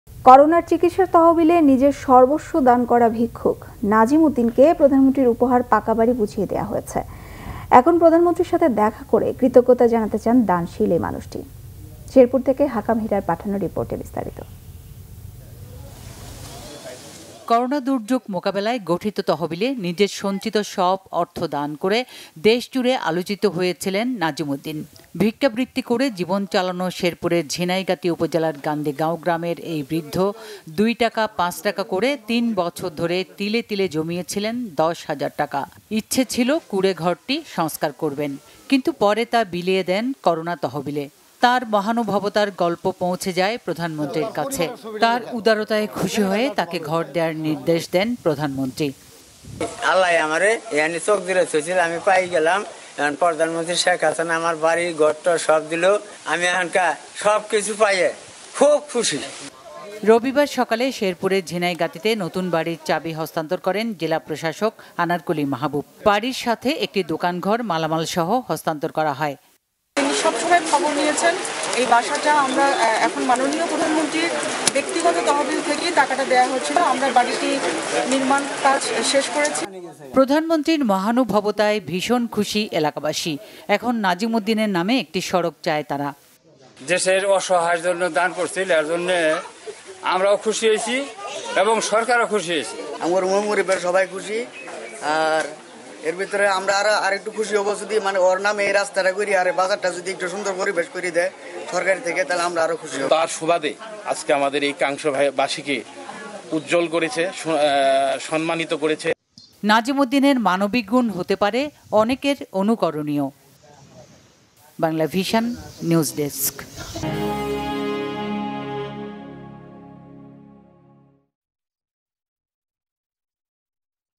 ভিক্ষুক নাজিম উদ্দিনকে প্রধানমন্ত্রীর উপহার পাকাবাড়ি বুঝিয়ে দেয়া হয়েছে | Banglavision News
*** ANTI-PIRACY WARNING ***